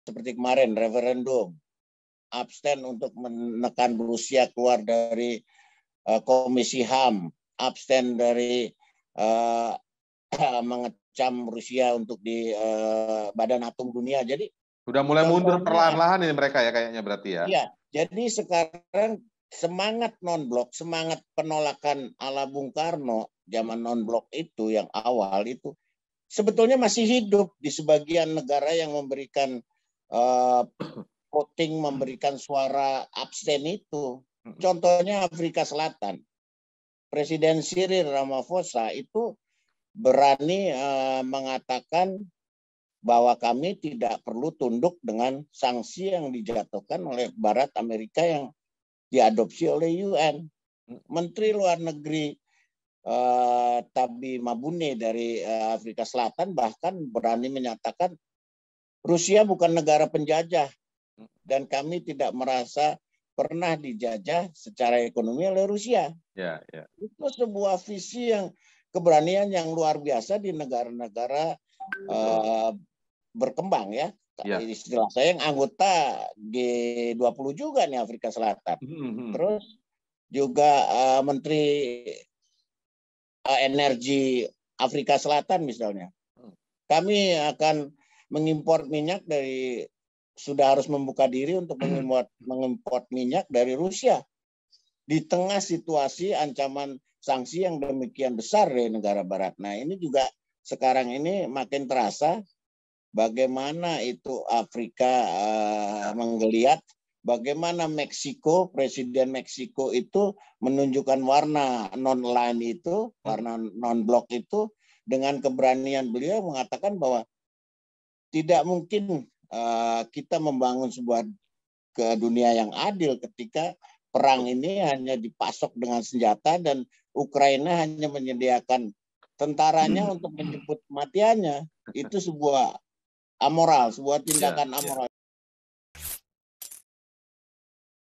SEMANGAT GERAKAN NONBLOK SOEKARNO BERGELORA DI NEGARA LAIN
Mantan Diplomat Indonesia Ple Priatna menilai gerakan nonblok masih relevan. Bahkan, negara-negara berkembang mulai lantang menyuarakan semangat tersebut. Ple mencontohkan tekanan agar Rusia keluar dari Komisi Hak Asasi Manusia (HAM) Perserikatan Bangsa-Bangsa (PBB). Sejumlah negara abstain salah satunya Afrika Selatan yang mana Presidennya Cyril Ramaphosa berani mengatakan kami tidak perlu tunduk dengan sanksi yang dilakukan Amerika Serikat dan Barat. Keberanian lainnya disuarakan Menteri Luar Negeri Afrika Selatan Grace Naledi Mandisa Pandor. Grace menyatakan Rusia bukan negara penjajah dan Afrika Selatan tidak pernah merasa dijajah secara ekonomi oleh Rusia. Tidak hanya itu, Menteri Energi dan Sumber Daya Afrika Selatan Gwede Mantashe menegaskan negaranya akan tetap mengimpor minyak dari Rusia.

Langkah itu berbeda dengan negara-negara Barat yang ogah mengimpor dari Rusia. Semangat nonblok juga disampaikan Presiden Meksiko Andres Manuel Lopez Obrador. Andres sempat mengatakan tidak mungkin perdamaian dunia tewujud ketika ada perang dan ada pasokan senjata.